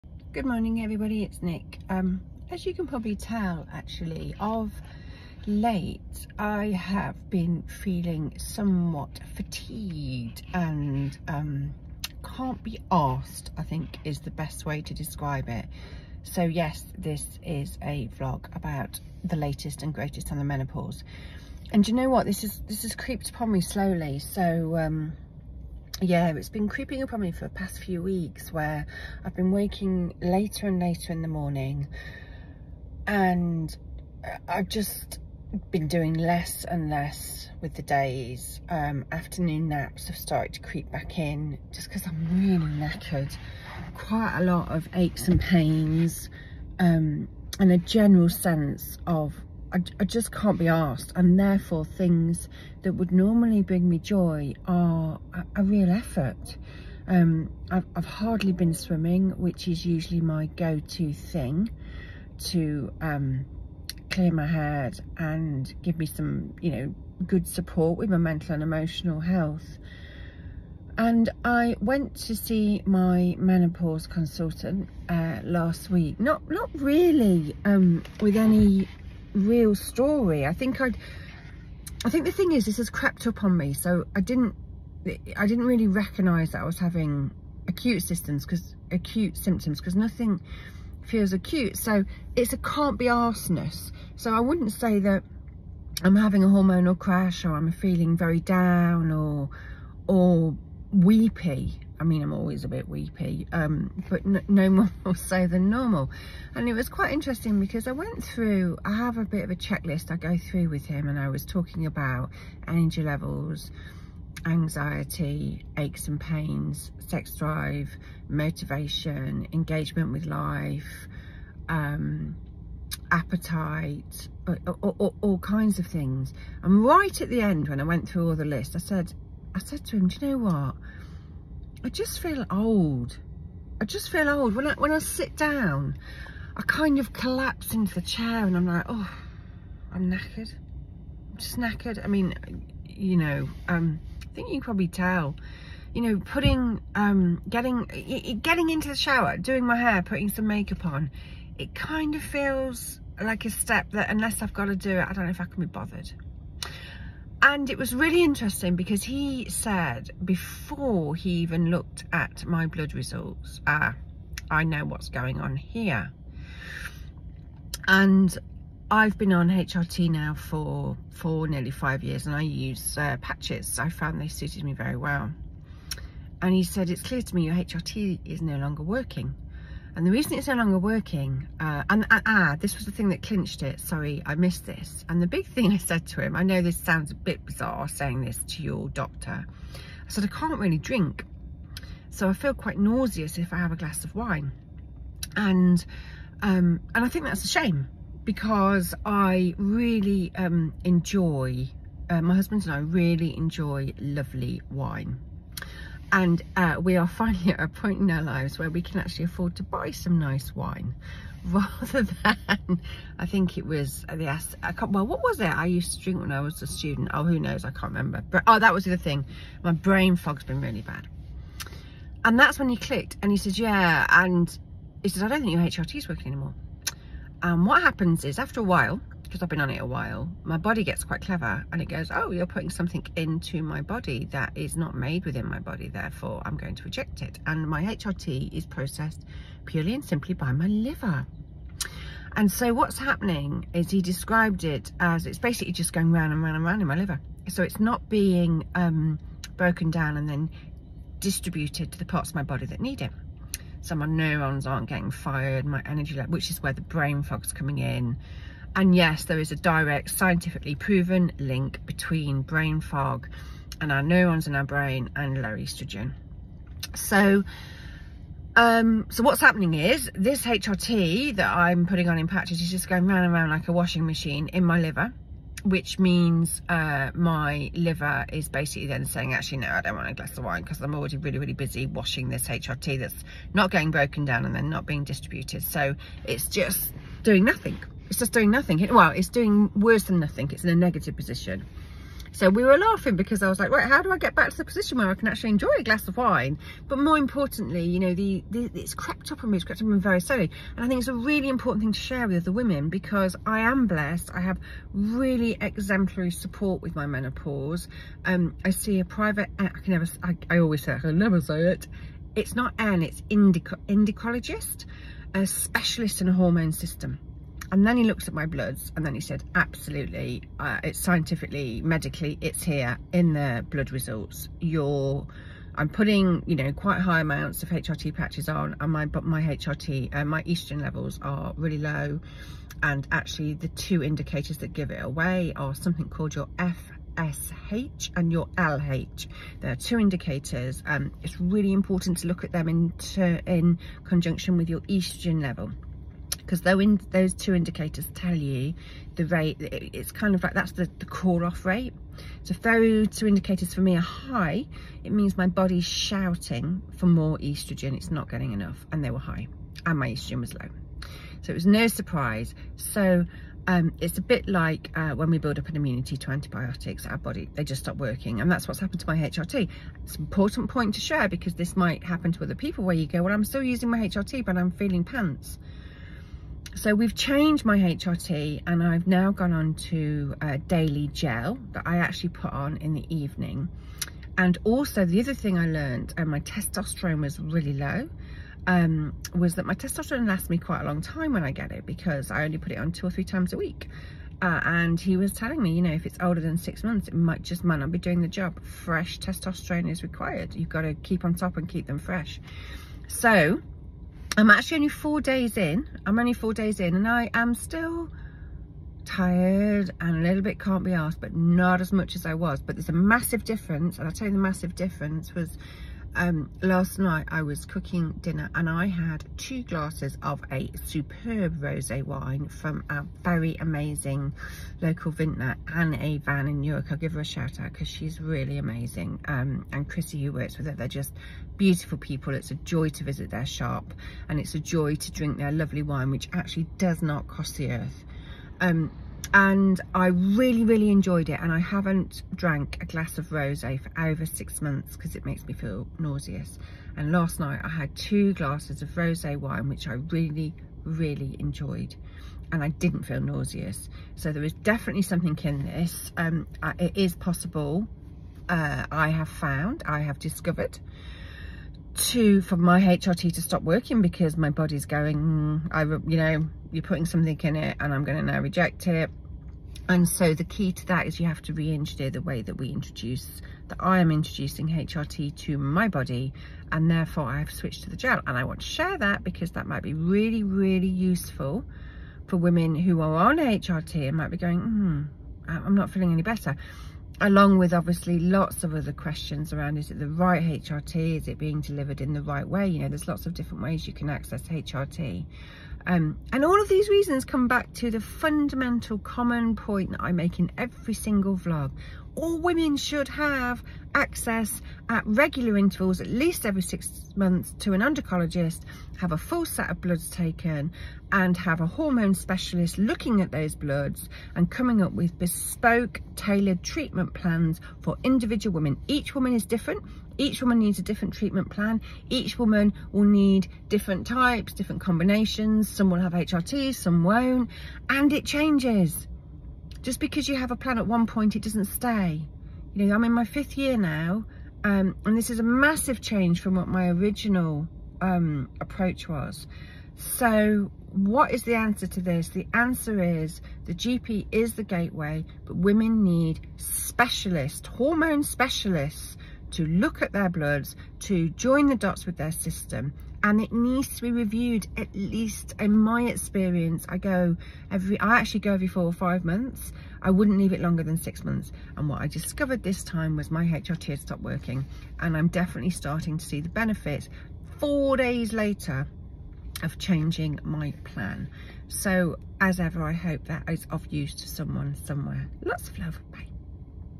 Tachyphylaxis and HRT - how my HRT stopped working and what I can do about it
So, tachyphylaxis is real and I've discovered, it can and is affecting the effectiveness of my HRT.

For those of you, who like me, have not heard of tachyphylaxis before, it is defined as:

"rapidly diminishing response to successive doses of a drug, rendering it less effective"

As a result, my HRT patches are no longer working, I have been on them for over 4 years and my liver is simply churning the oestrogen round and round without breaking it down into it's component parts and distributing it to the parts of my anatomy that need it.

And the result of this, is all of my old symptoms are back: fatigue, brain fog, cannot be arsed syndrome, aches and pains and a general feeling of just 'being old'.

The solution is to change the way I introduce the HRT into my body, so I have switched from my patches to gel, it's early days but at least I no longer feel nauseous after drinking a small glass of wine (nausea is the way my liver communicates with me, it was telling me that it was a tad busy with other stuff, i.e. the un-processed oestrogen).

And again, this latest development on my journey through the menopause further convinces me of the need to ensure that all women presenting with menopausal symptoms get access to a specialist (I pay to see a private endocrinologist, as the NHS does not, at this time, provide wide enough access to them ), at regular intervals - at least every 6 months. Our bloods need to be analysed by people who are trained to do so, it is not enough to simply look at hormone levels in isolation and only compared to vast averages.

Because the menopause does not stand still, we do not stand still and our needs change.

So, again I urge the health service to look at providing much more accessible endocrinology support as well as emotional and therapeutic support to all menopausal women - in the long term, I'm sure it will be hugely preventative for many symptoms, life saving for some women and life changing for others .

Slapping on HRT and telling us to get on with it, is absolutely not an acceptable way forward.

With love

Nik x